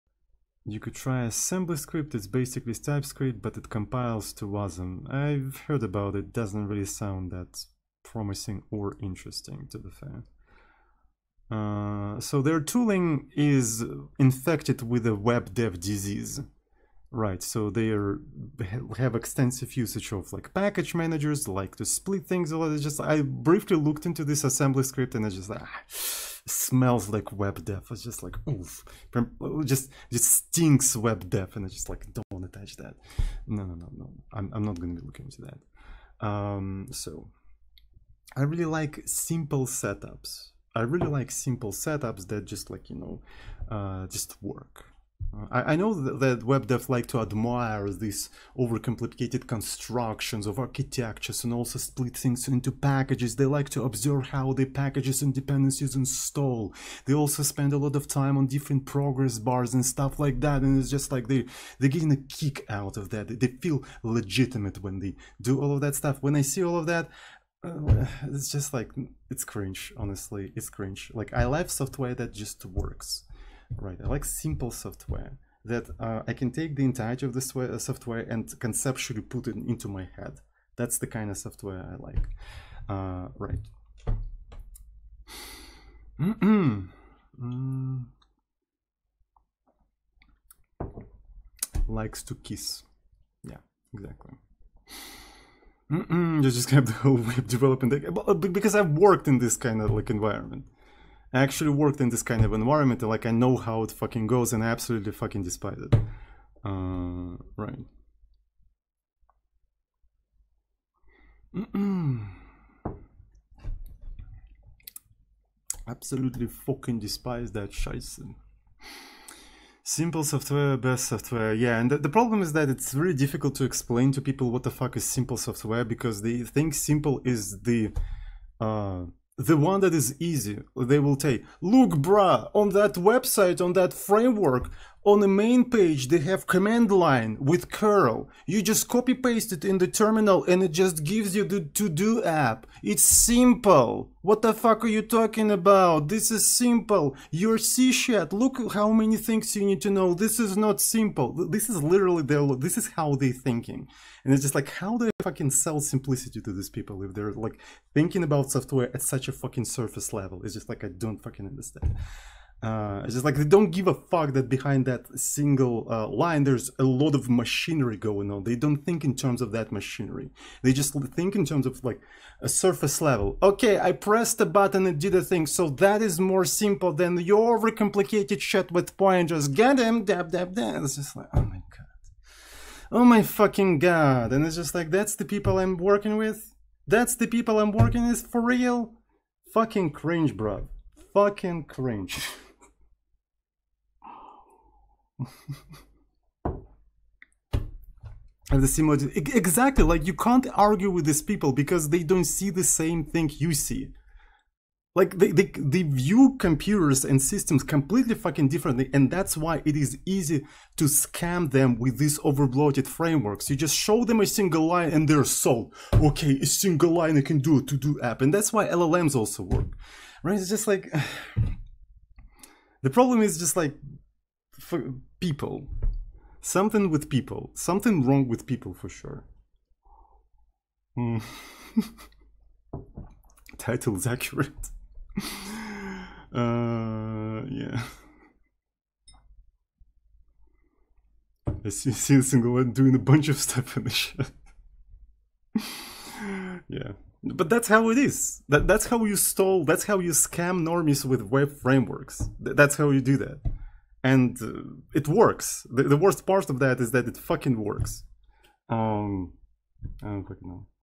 Tsoding about Simple Software
He mainly discuss the notion of simple software.
From the "I'm Already Losing It" Stream.

Don't quite remember but the code in the thumbnail was I think from one of Tsoding's posts on Twitter but I am not sure.